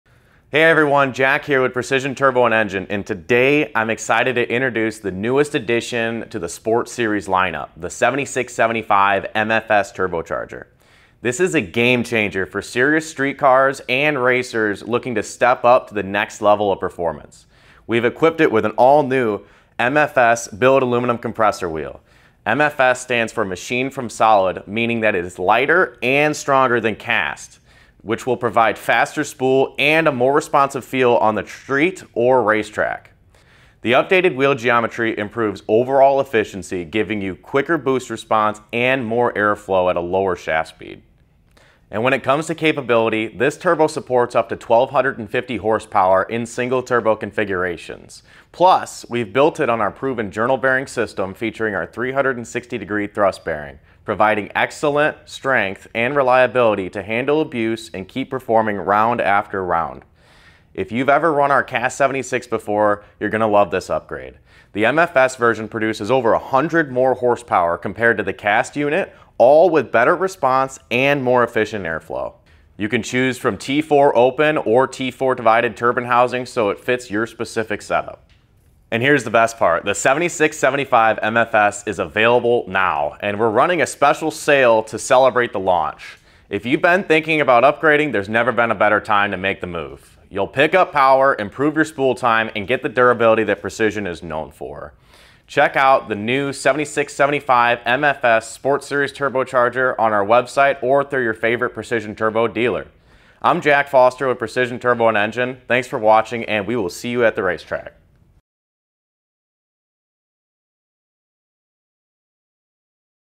7675 MFS Sport Series Turbocharger - The most affordable 1250 horsepower turbocharger in the market
Say hello to the next evolution in performance — the 7675 MFS small frame turbo is here, and it’s bringing serious upgrades over the original CAST 7675.

Featuring:
- New billet aluminum compressor wheel = reduced rotating mass + improved airflow
- Updated geometry for quicker spool + better efficiency
- Supports up to 1,250 HP in single turbo setups
- Built around our proven 360° journal bearing system for superior durability under pressure
- Engineered for enthusiasts who demand more — without the price tag of full race-spec parts — the Sport Series is your go-to for reliable, high-output boost.